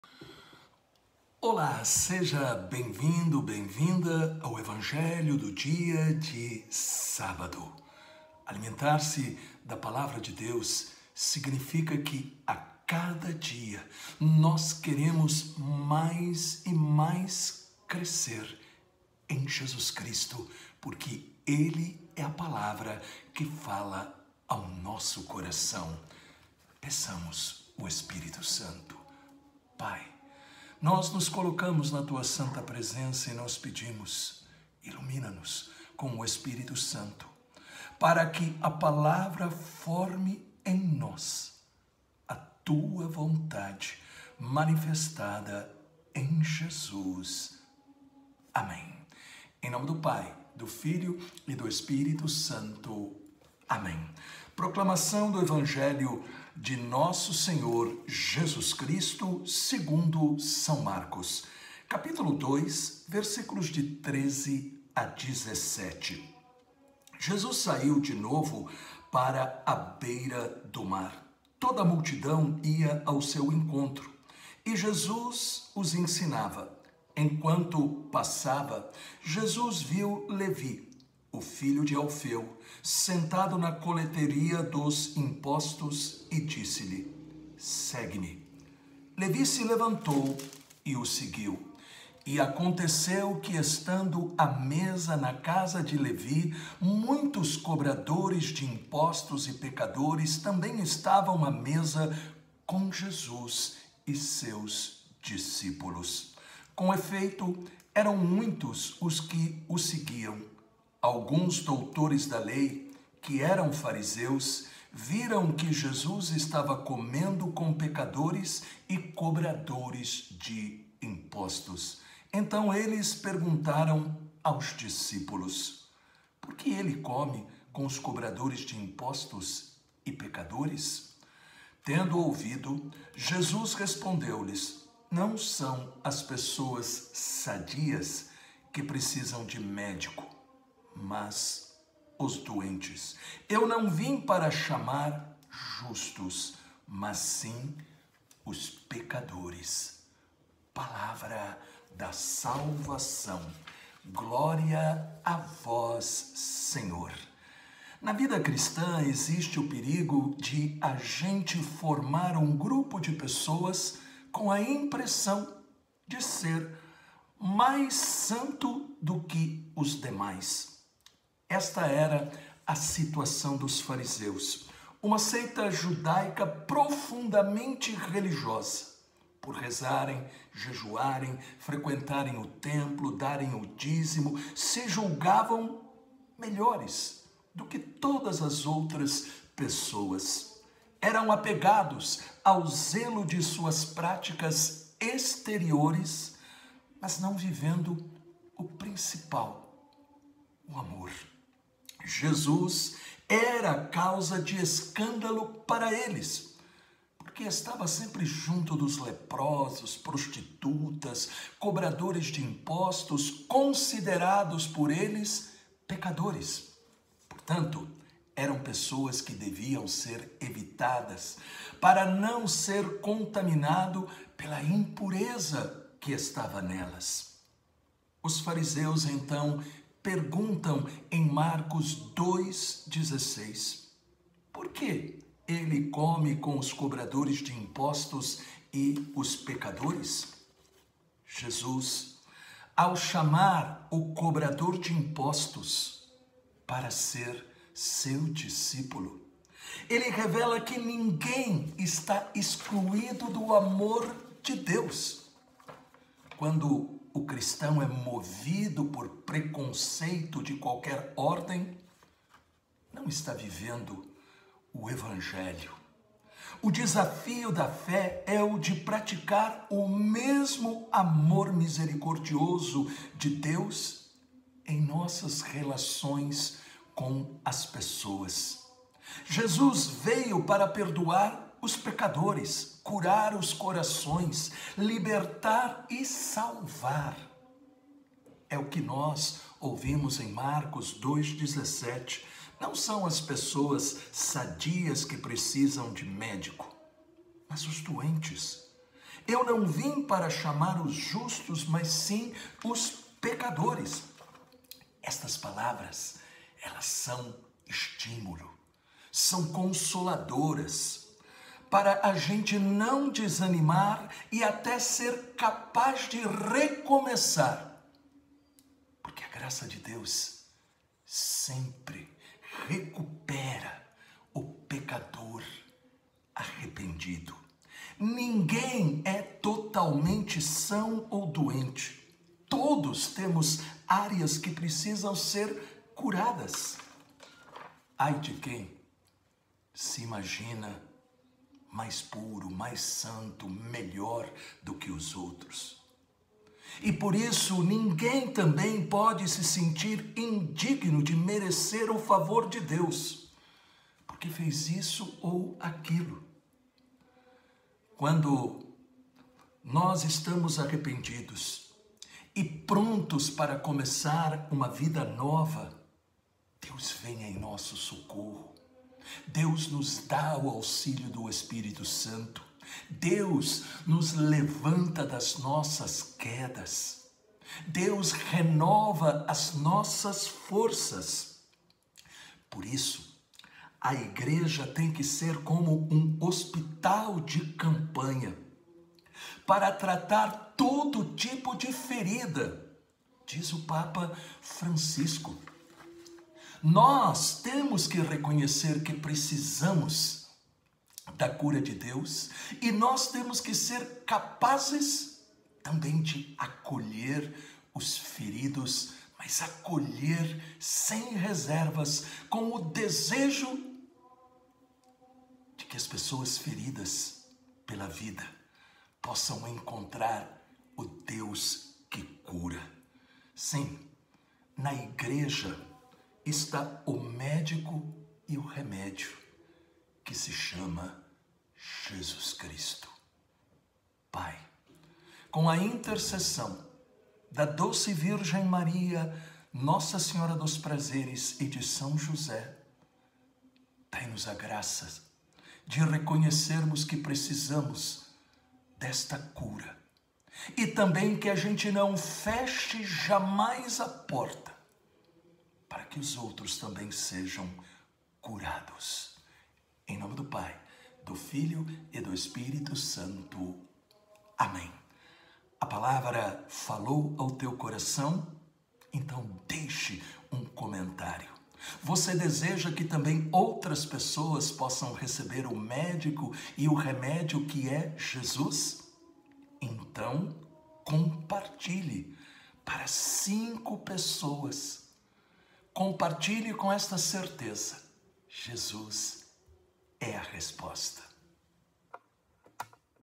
EVANGELHO DO DIA: 16/01 sábado: CURA-ME JESUS! Mc 2,13-17 - Padre Alberto Gambarini
Ao chamar o cobrador de impostos, Jesus, revela que ninguém está excluído do amor de Deus. Deixe um comentário e compartilhe para 5 pessoas que Deus alcançará através de você.